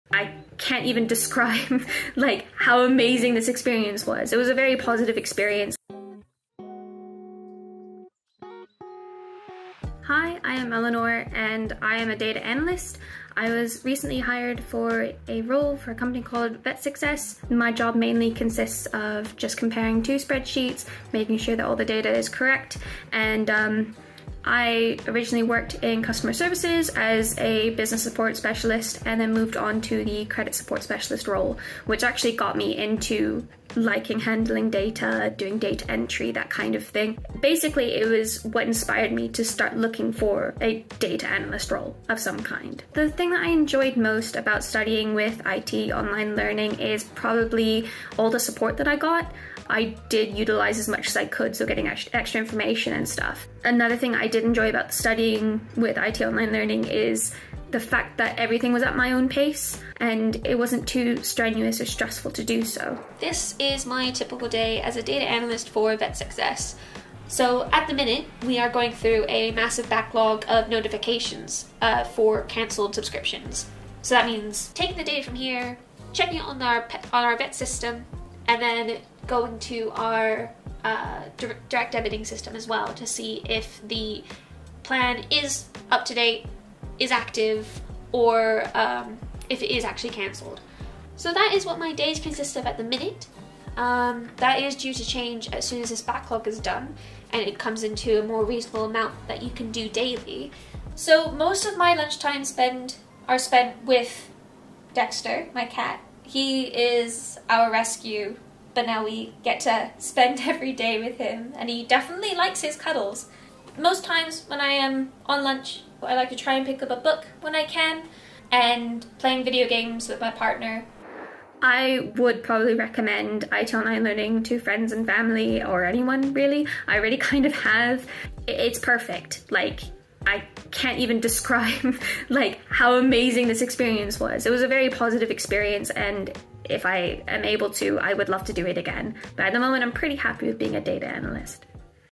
Pursuing Her Passion for Data Analytics & Landing the Ideal Role | Eleanore's ITonlinelearning Story
*A Day In The Life Of A Data Analysis Graduate*

Eleanore shares her incredible journey with ITonlinelearning. After enrolling in the Data Analyst Programme, she earned key certifications in CompTIA Data+ and Business Analysis Foundation. With our supportive recruitment team, she received a CV and LinkedIn update, job search assistance and interview preparation. Just four weeks into her recruitment journey, Eleanore secured a Data Analyst role. She credits ITonlinelearning for helping her successfully transition into the data industry and recommends the programme to anyone looking to upskill and change careers.

🔗 Join Eleanore and start your journey in Data Analysis with ITonlinelearning today